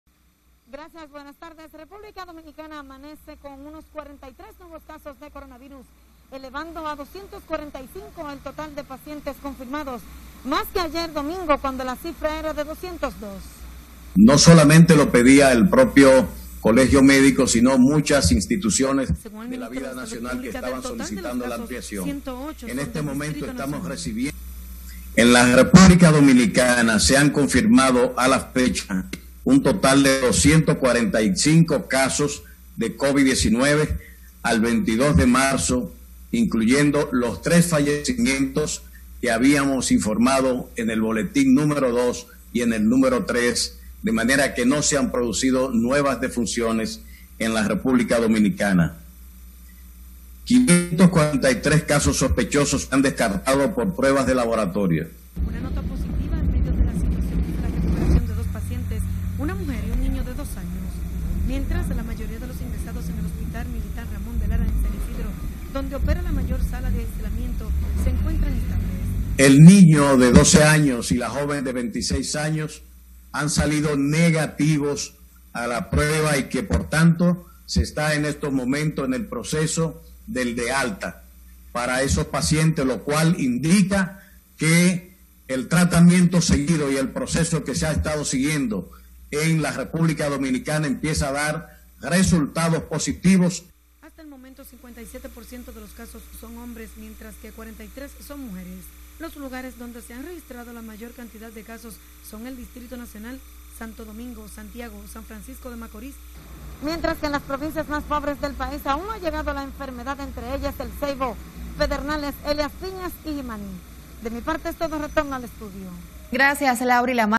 Aumentan casos coronavirus; el Distrito, Santiago y San Francisco los más afectados
Los infectados con coronavirus se elevaron a 245 al registrarse 43 casos nuevos del virus que sigue concentrado en la capital, Santo Domingo, Santiago y San Francisco de Macorís.